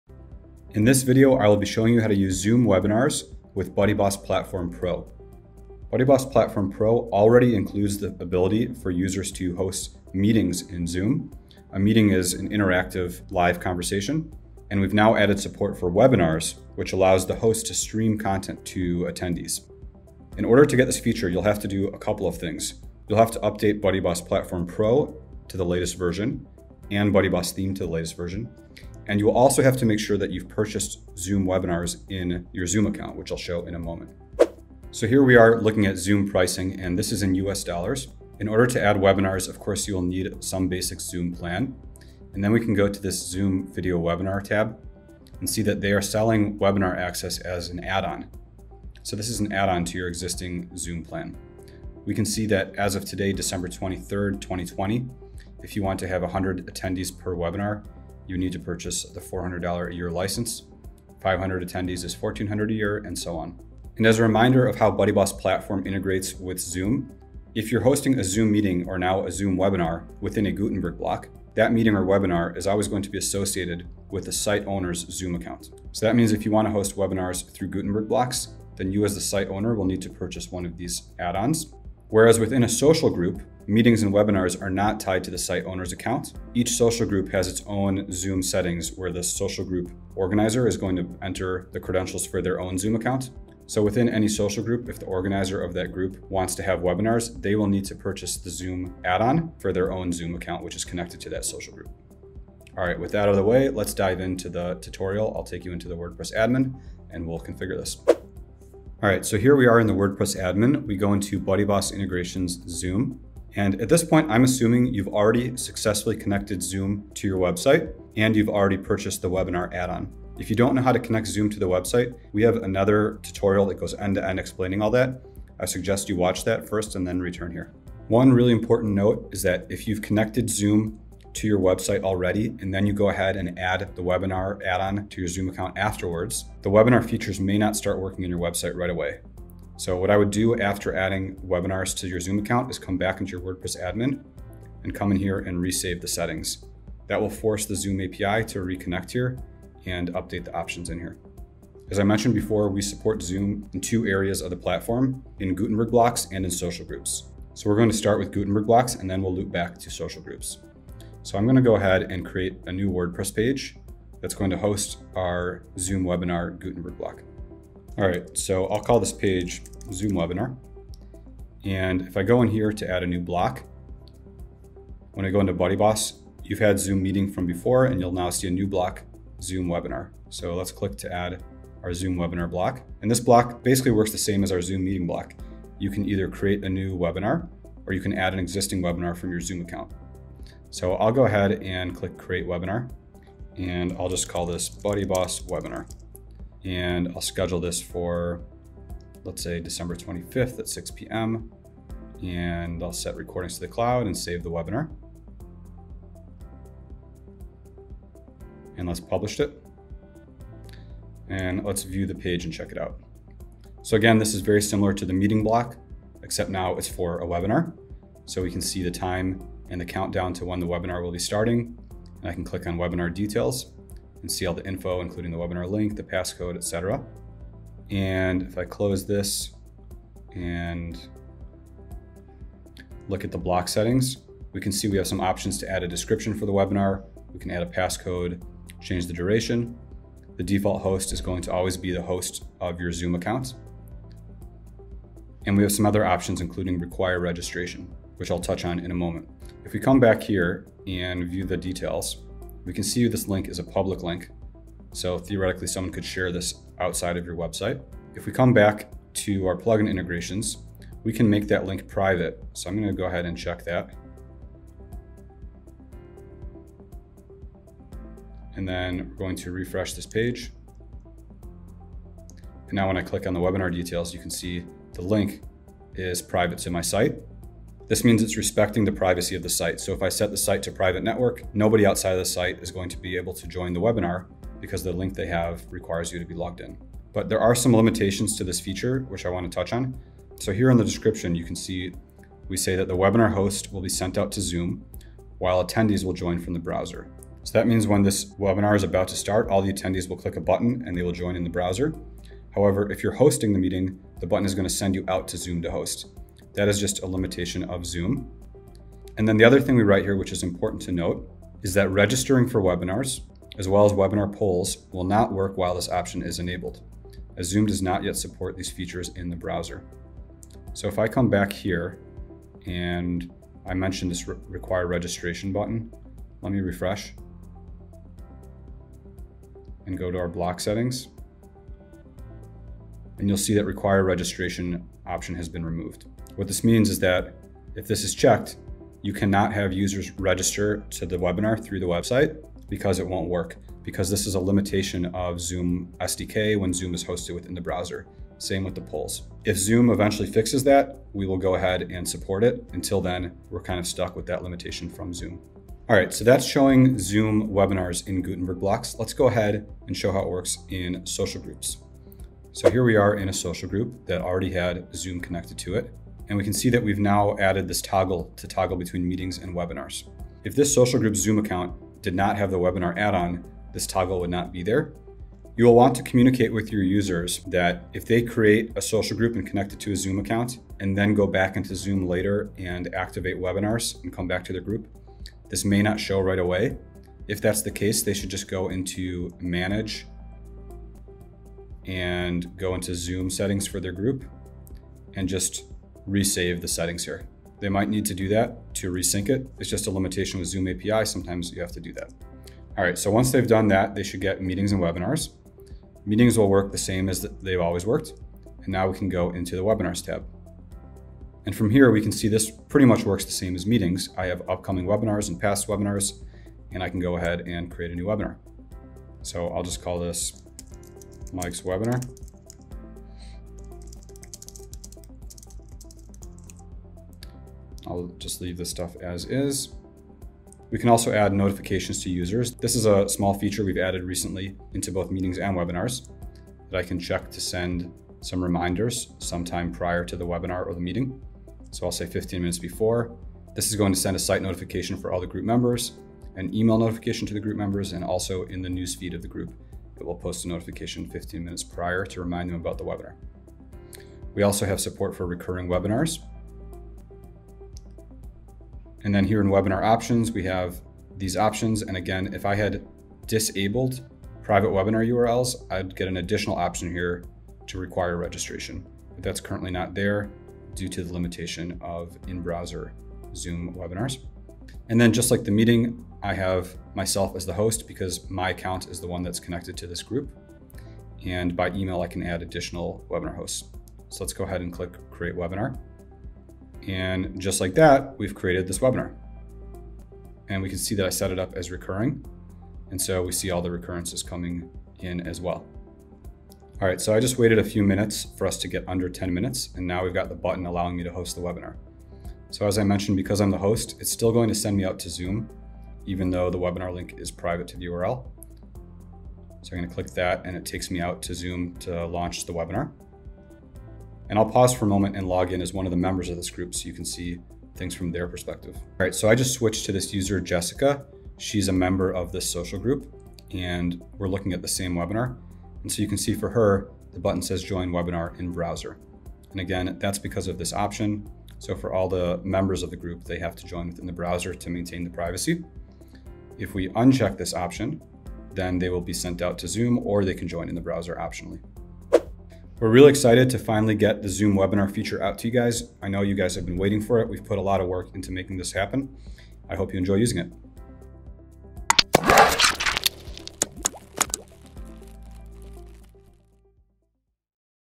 How to use Zoom Webinars | BuddyBoss Platform Pro
In this tutorial, you will learn how to use Zoom Webinars which is a new addition to our Zoom integration.

Zoom Webinar option can be used both in Gutenberg blocks and in social groups. This feature requires BuddyBoss Platform Pro.